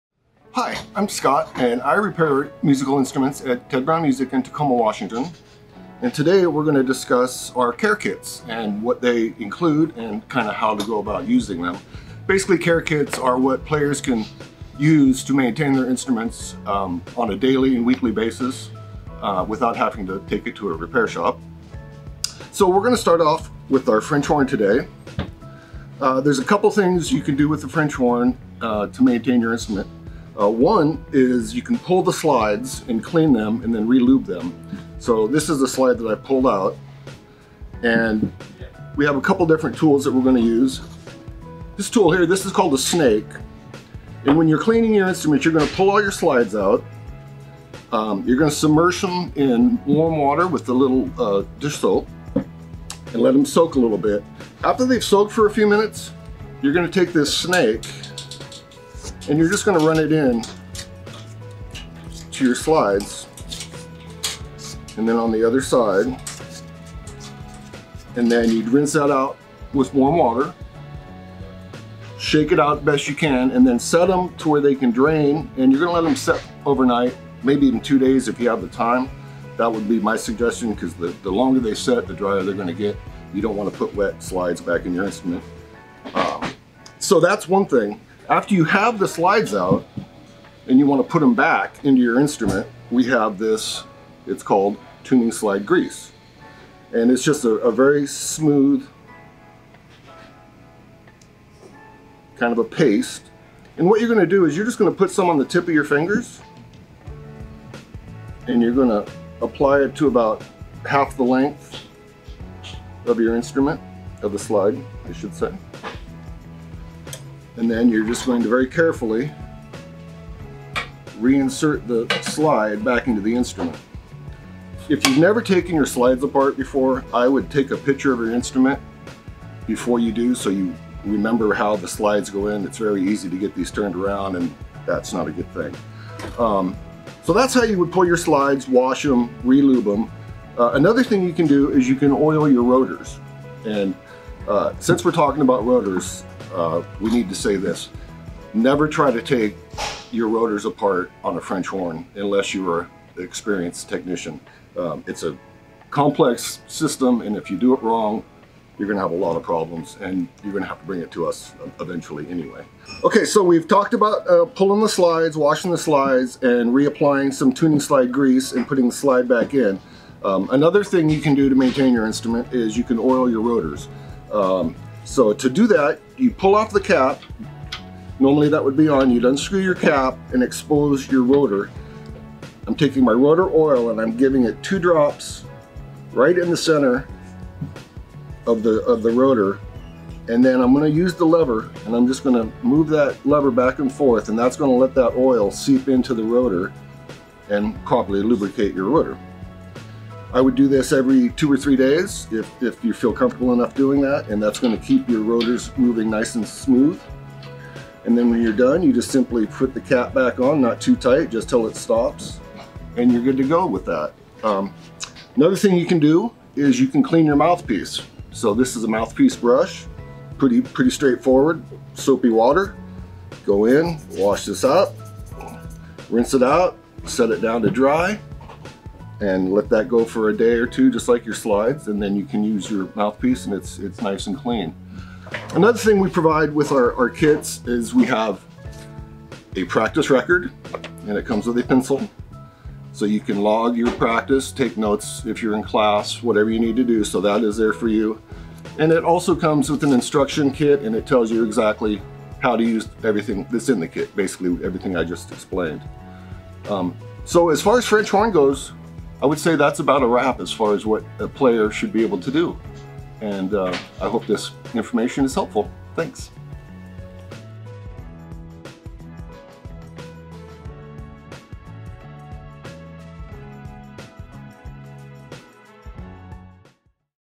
French Horn Care Kit
A Ted Brown Music walkthrough of our French Horn Care Kit with a demonstration from one of our repair techs.
Visit us in-store or online at tedbrownmusic.com